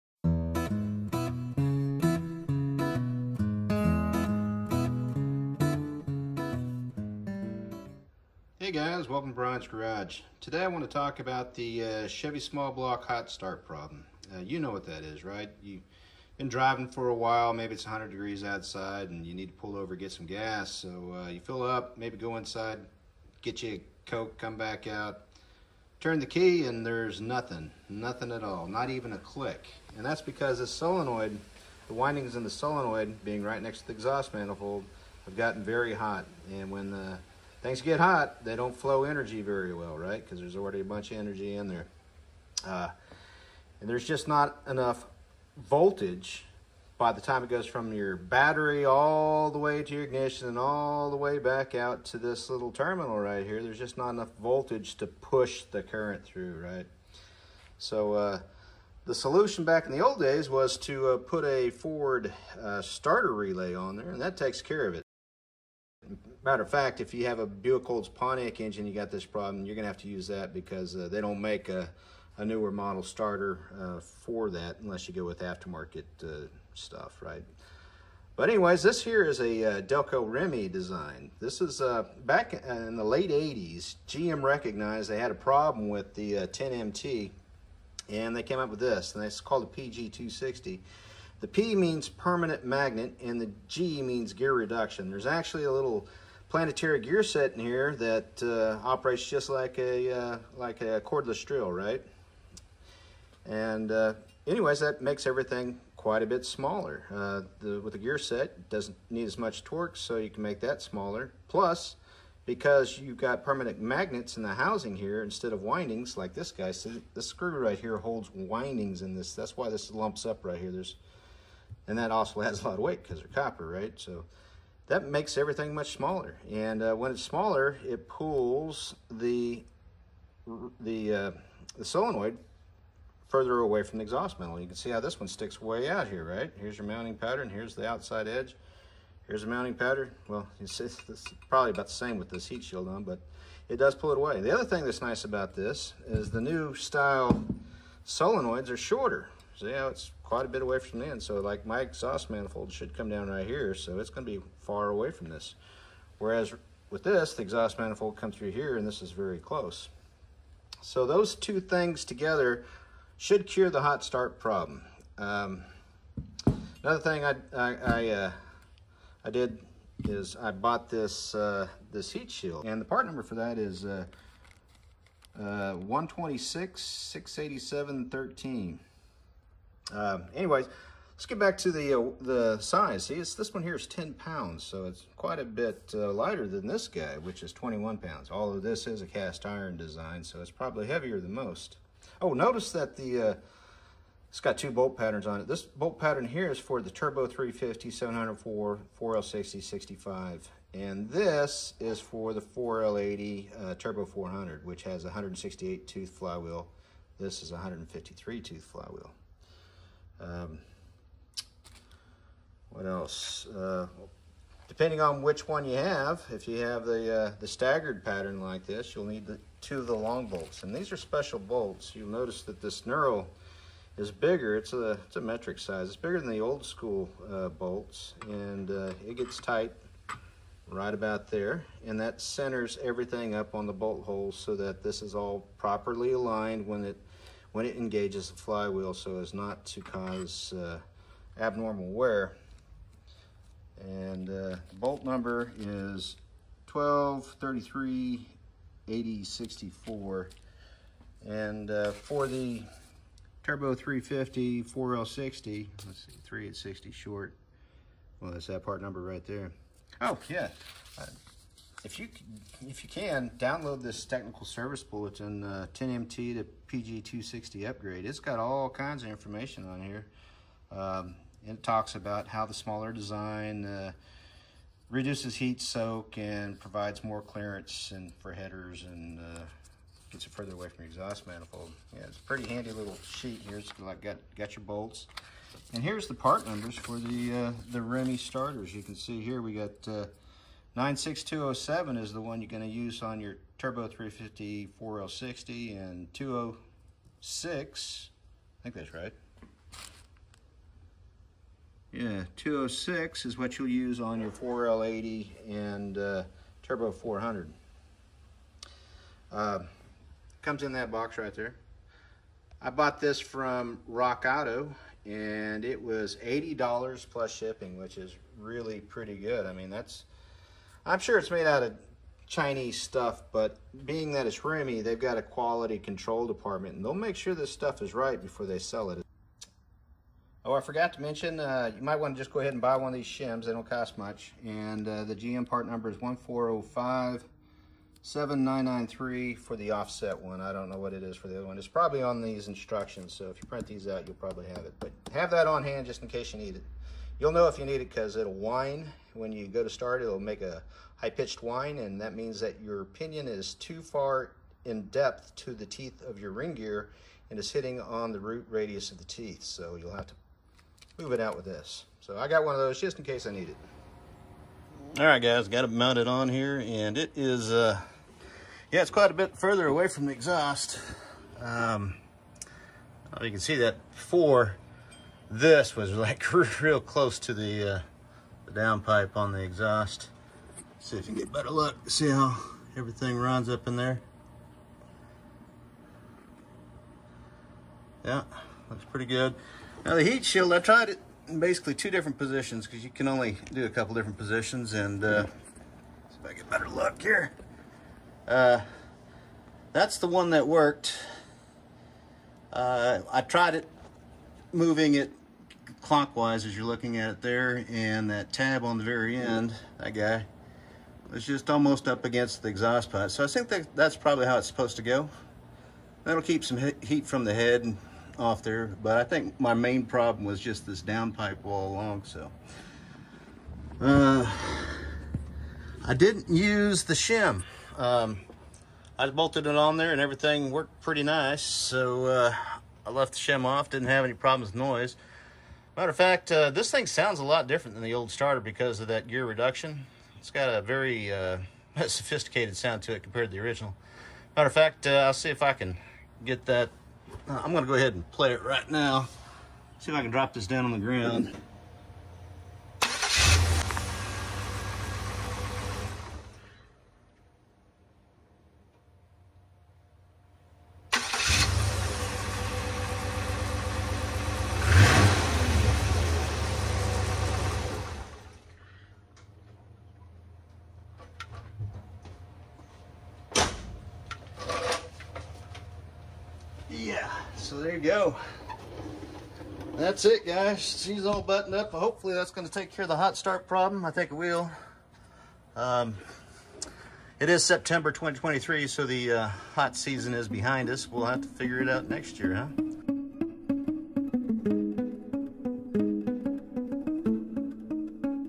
Best Chevy Hot Solenoid Fix! AND it sounds too cool....
We use a modern Remy PG260 to fix an old school Chevy HOT no-start problem.
Owing to a permanent magnet and gear reduction design, it is much smaller and farther away from the exhaust manifold.
It also sounds way cooler than a direct drive starter.
Costs $80 at RockAuto (9/23/2024).